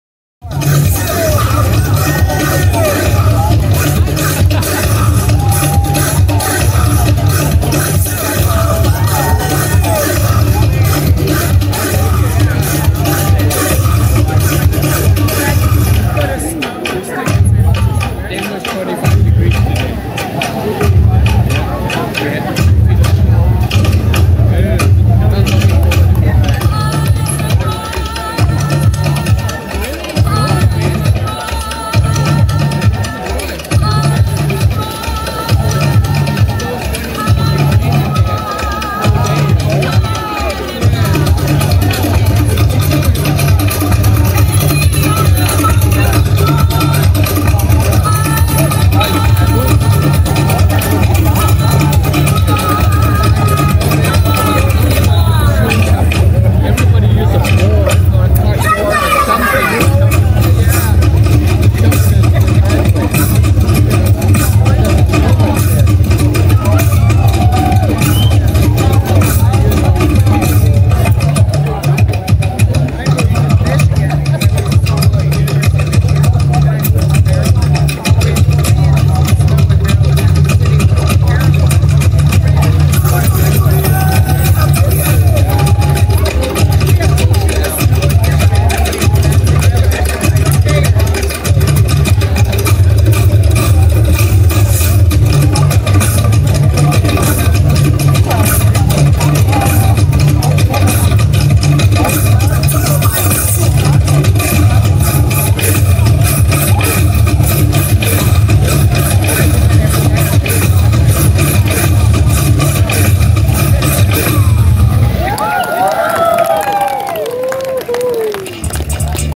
Hula dance, beach party - Maria, entertainement staff.
Maria  doing the Hula, Villa Del Palmar Flamingos - March 24, 2016
Samsung  Galaxy S5 phone